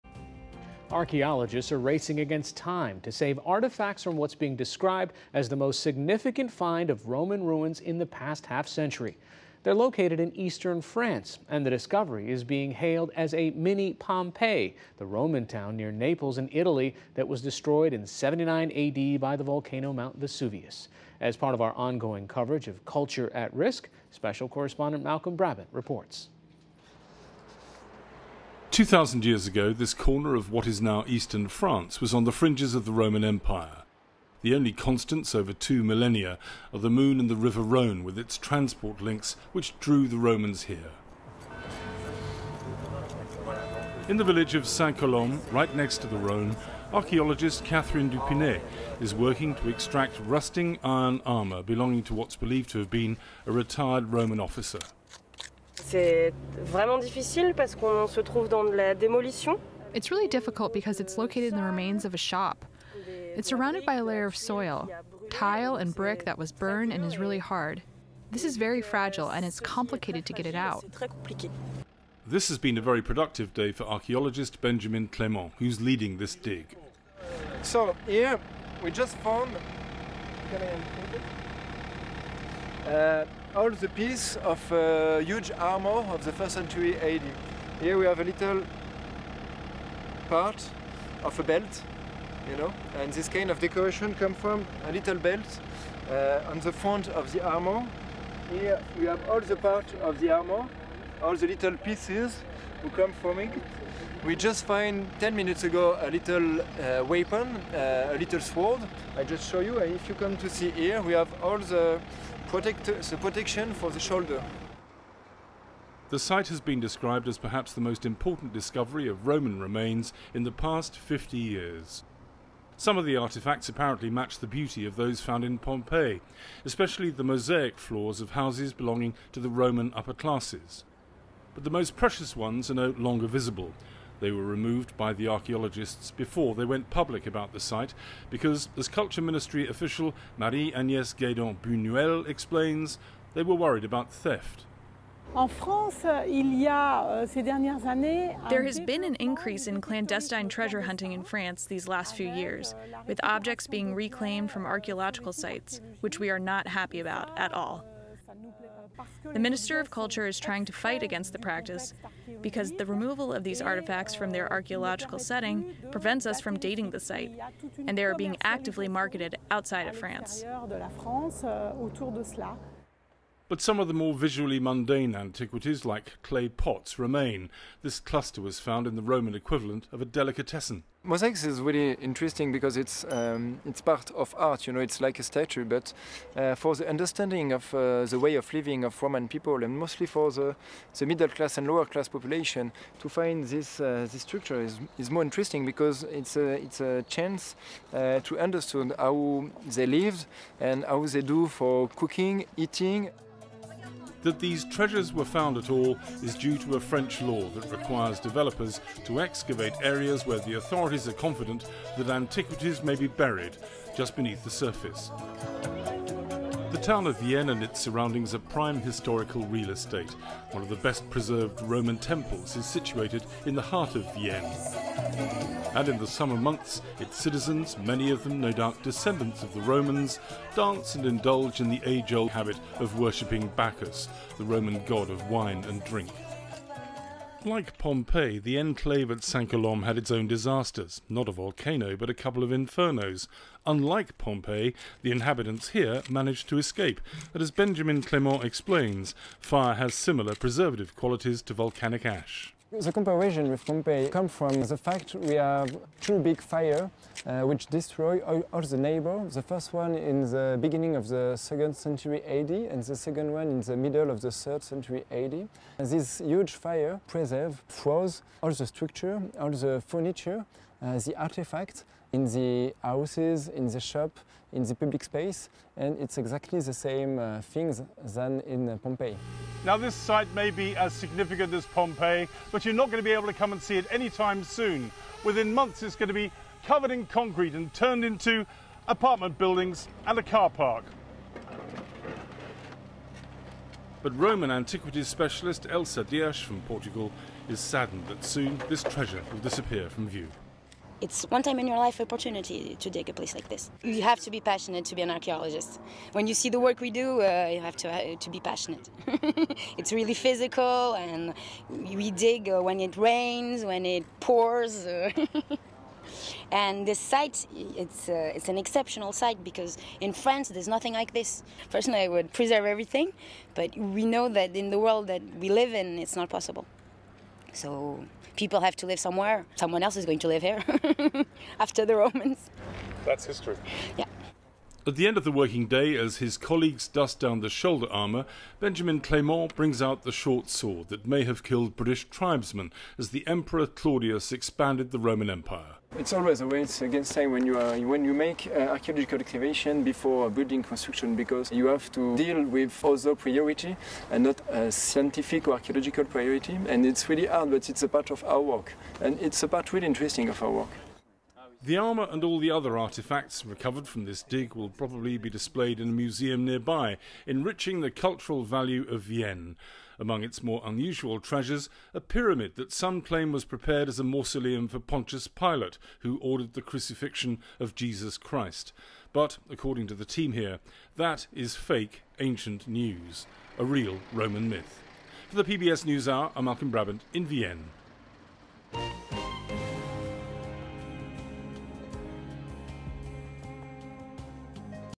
Archeologists dig up France’s ‘mini Pompeii,’ a Roman town frozen in time by fire
There’s an archaeological race against time underway in France as archaeologists work to save artifacts from a site dubbed as a “mini Pompeii.” This discovery of these 2,000-year-old Roman ruins is being called perhaps the most important of its kind in the past 50 years. Special correspondent Malcolm Brabant reports.